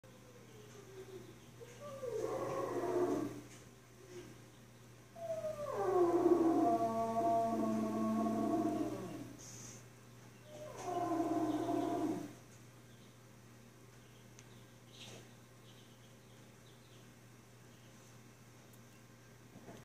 Bear 4/19/2017 b
Bear is not happy. He was just neutered and confined to the patio. His howl sounds more like a bear than  dog! I guess we selected the right name for him LOL